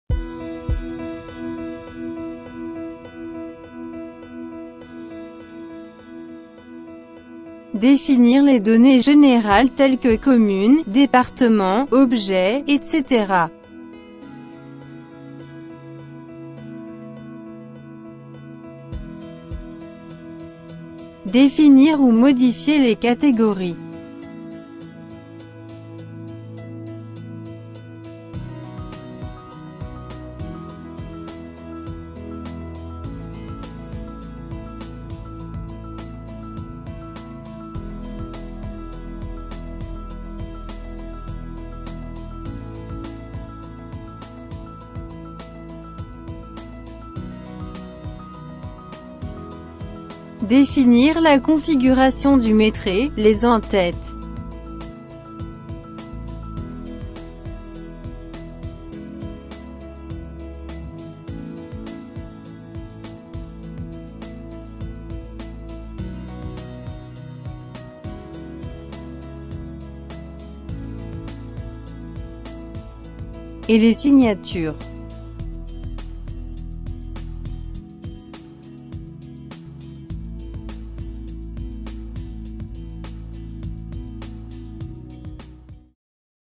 Tutoriel Vidéo de PriMus IFC - Les données générales - ACCA software
Comment définir les données générales du métré ? Regardez comment définir les données générales du métré avec PriMus IFC, le logiciel IFC BIM 5D de Métré.